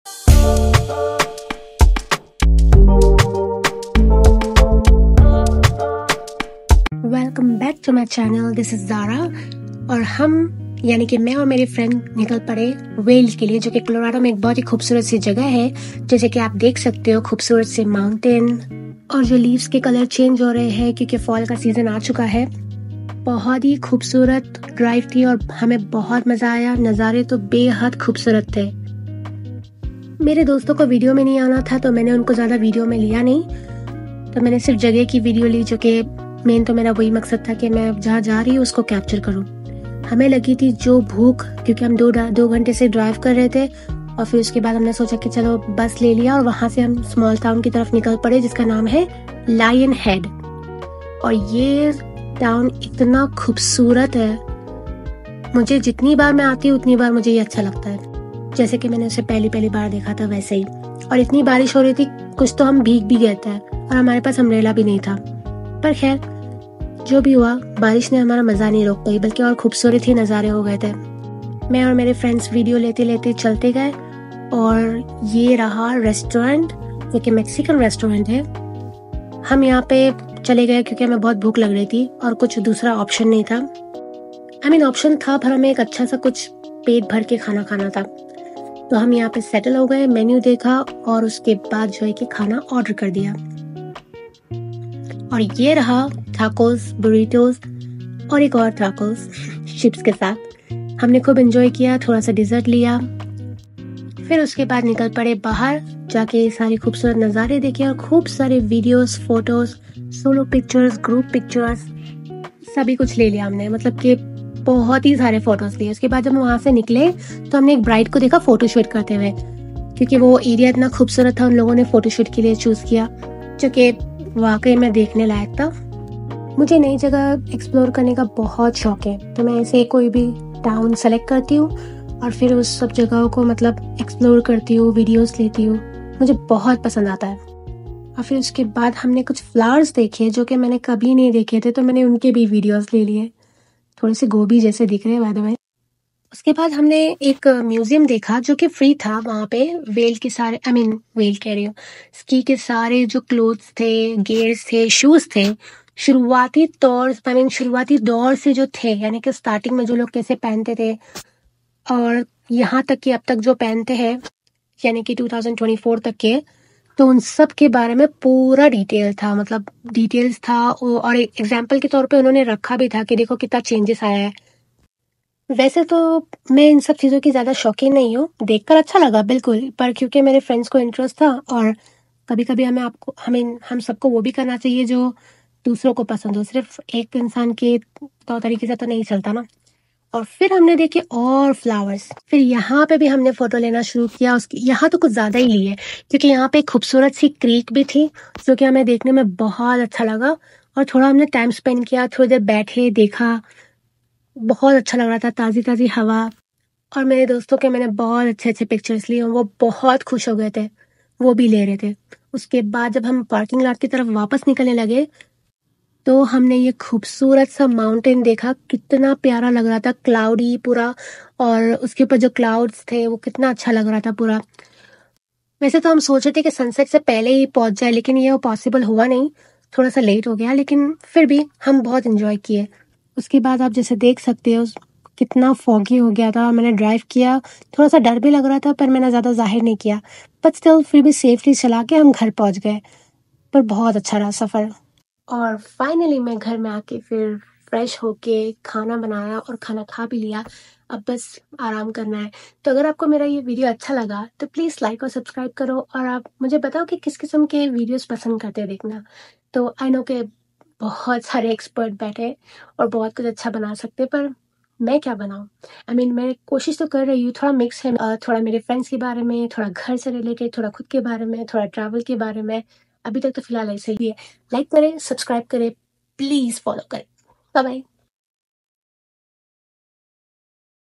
Exploring Vail’s Charm: Town Life and Scenic Routes.
Join us as we take a scenic drive through Vail, enjoy a delicious lunch, and explore both the beautiful town and its stunning surroundings. A perfect day filled with great views and memorable experiences!